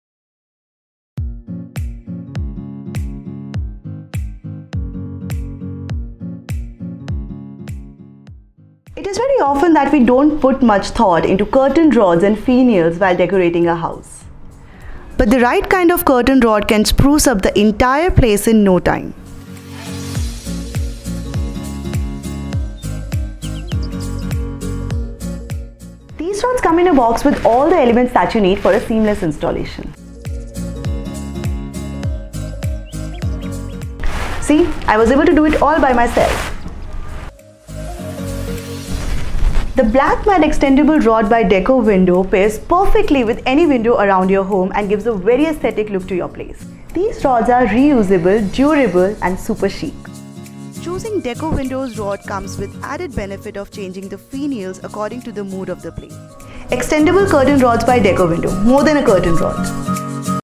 Curtain Rod or Drapery Rod- Perfect window décor element!
Curtains rods are one of the most important elements while considering window décor. The right rod selection will not only enhance the over all look of the space but also functionally add to the design.

Decowindow offers a wide collection of Curtain Rods, These extendable rods are a perfect fit to any window and the best part about them is...when you consider a house change, these rods move with you!

 Features:
* Extendable rods available in 36''-66'' & 52''-144''
* Multiple color options available
* Load capacity- up to 25kg
* Designer Finials
* Available in double rod set